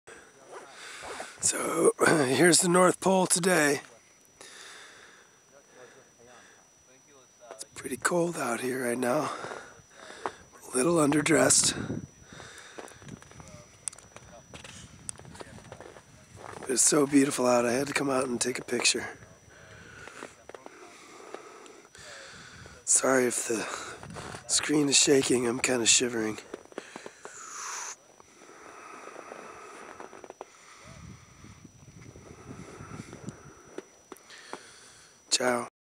Geographic North Pole at Midnight on April 14, 2011
This is what the Geographic North Pole looked like on April 14, 2011 at midnight. We arrived by ski a few hours earlier and I jumped out of the tent to grab a quick panoramic video. My voice is messed up from a respiratory infection.